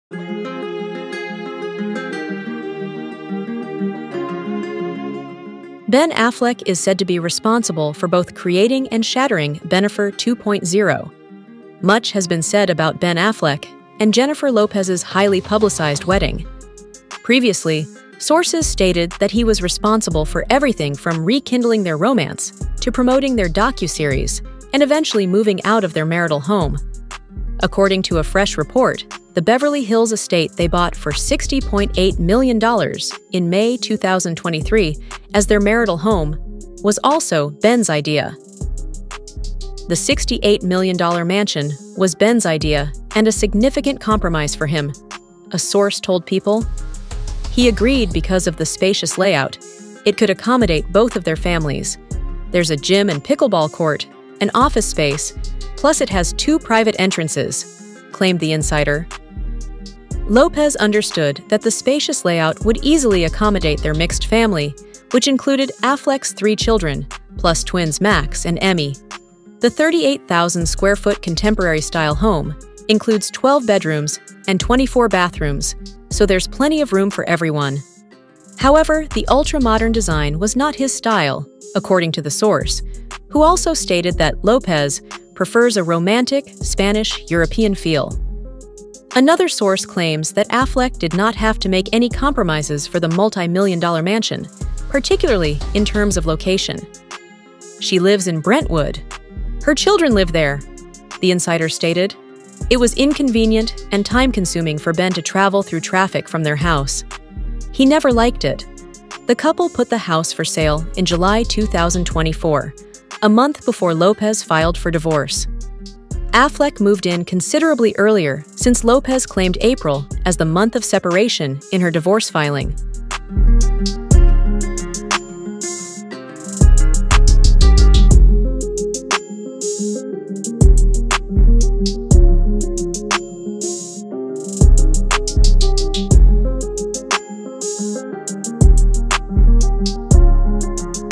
Ben Affleck forced Jennifer Lopez to make big sacrifices before filing for divorce
Ben Affleck is said to be responsible for both creating and shattering Bennifer 2.0. Much has been said about Ben Affleck and Jennifer Lopez's highly publicized wedding.